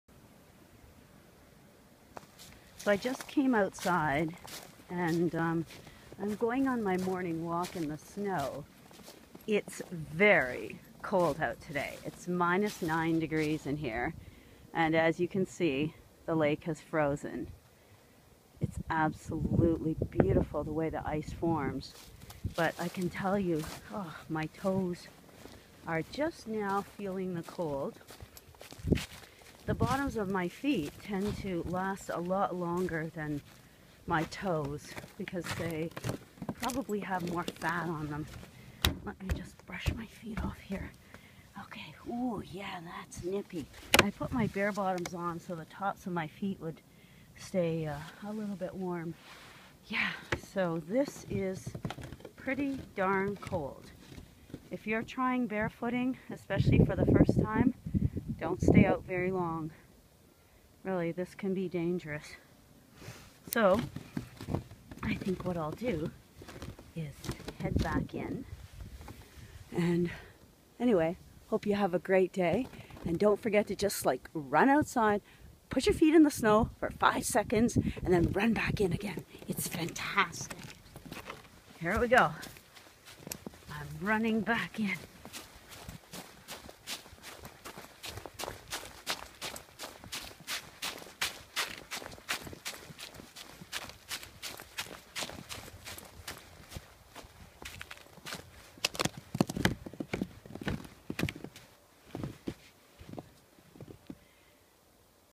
How to Wear Barefeet in Freezing Snow
It was 9°C this day and we had another snowfall overnight. Don't do this at home for more then a few seconds unless you've been building up to it. I lasted about 2 1/2 minutes outside in the snow. I'm wearing Barebottom Shoes to keep the top of my feet warm. I could've gone longer but it's early in the snow season. I'd rather get my feet and all the proprioceptors accustomed to the cold temperatures. You'll see how my feet start to turn pink around the edges and white in the centre. There are sensory receptors on the soles of your feet that are constantly sending a message to the central nervous system to adapt the body to the cold environments. But the priority in the body is to keep the main organs warm. So if there isn't enough heat in the body to get to the extremities, like your feet, that's when you get frostbite. When I came back in the house after about ten minutes my feet were a beautiful red pink colour filled with thinned blood rushing through them. I challenge you to go outside for 5 or ten seconds in the snow. It's life changing!!!